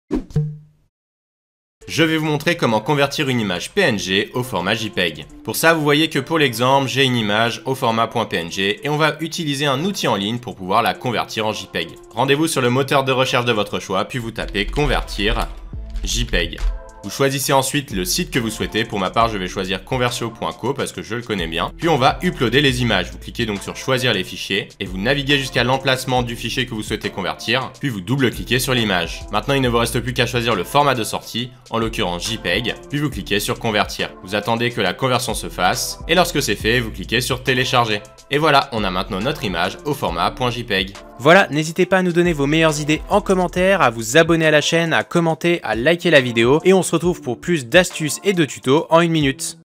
Convertir une image PNG en JPG
Apprenez à convertir une image PNG en format JPG, conversion de PNG en JPG, méthodes et outils pour changer le format d'image de PNG en JPG, guide de conversion d'images PNG.